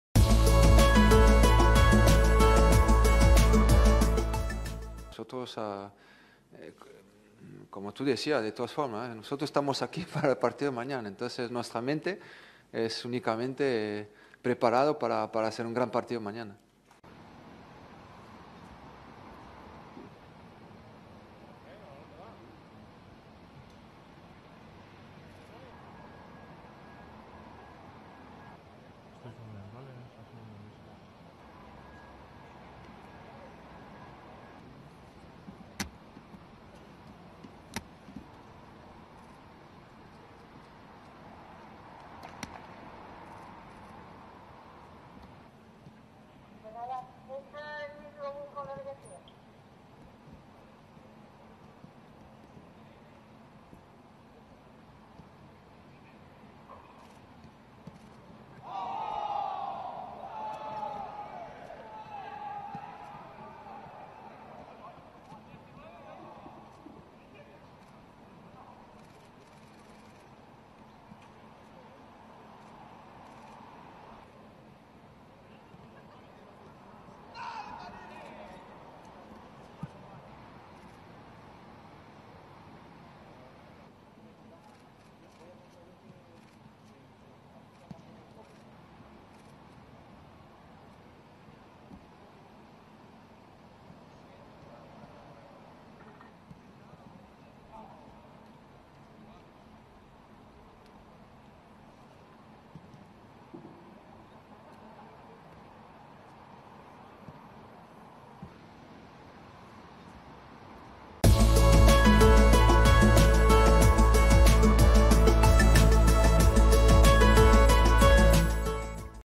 Zidane quiere la Supercopa: "Estamos preparados"
El Real Madrid comienza a defender ante el Athletic en Málaga el título de Supercopa. Zidane advierte que los blancos están "preparados" para lograr el primer título en juego de la temporada.